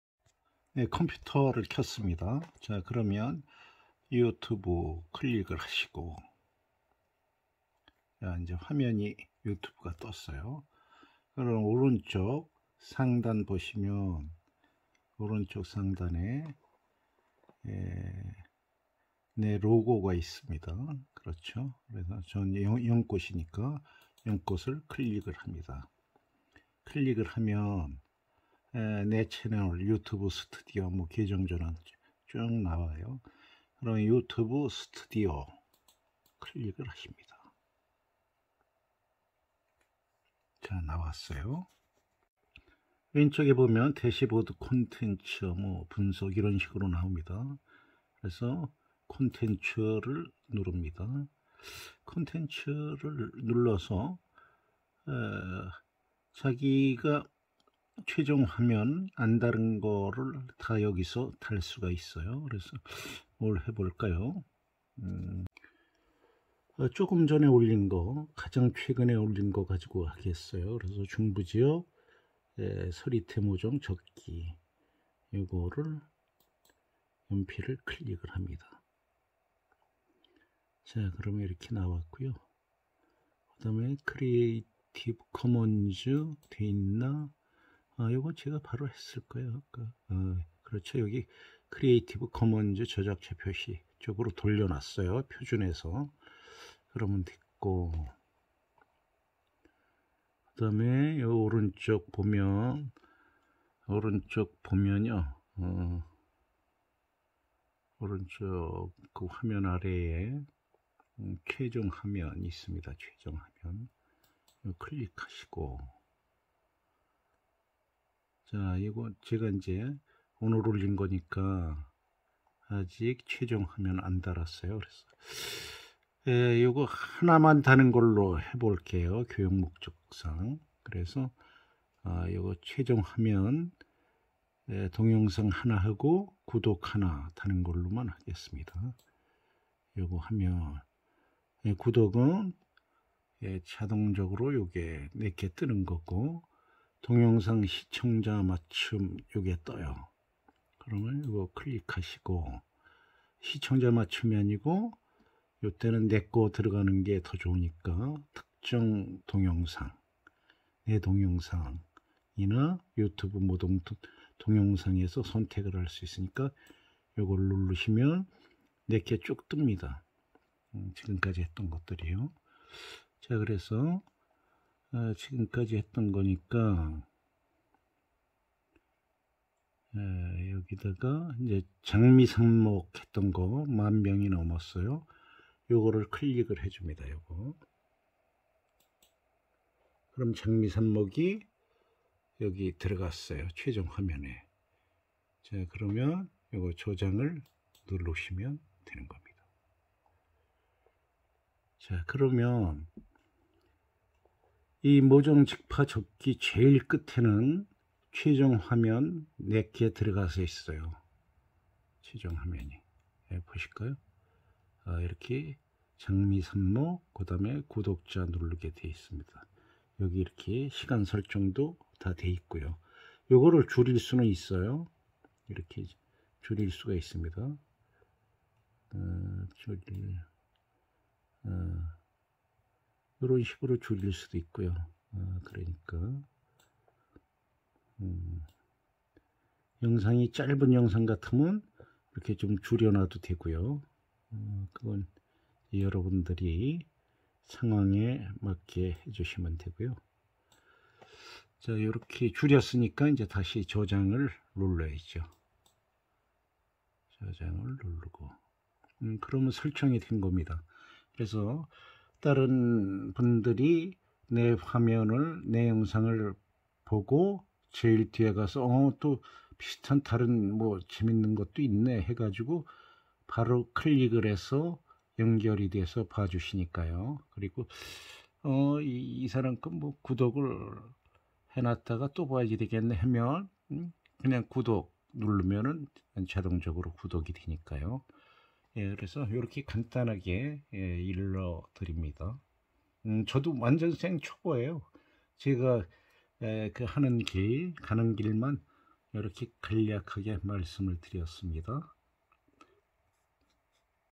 완죤초보가 왕초보에게 최종화면 넣는방법 알려주기 최종화면 영상끝날때 다음화면
영상올리고 최종화면 넣는방법을 모르시는분들이 초보유튜브분들은  대부분이실겁니다.
저도 스튜디오에서 하는길만 안답니다.아주 친한 지인에게 하는방법 영상 보내드리다가 촬영한것 그대로 올려봅니다..